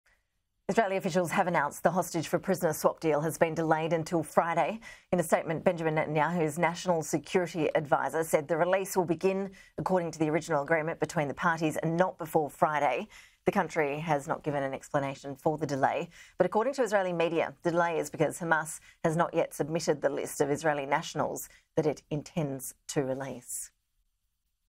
Israeli hostage release deal delayed until Friday
Israeli officials have announced the hostage-for-prisoner swap deal has been delayed until Friday.

Benjamin Netanyahu's national security adviser Tzachi Hanegbi released a statement on the swap.

The statement said, ‘The release will begin according to the original agreement between the parties, and not before Friday.’

The country has not given an explanation for the delay.

According to Israeli media, the delay is because Hamas has not yet submitted the list of Israeli nationals it intends to release.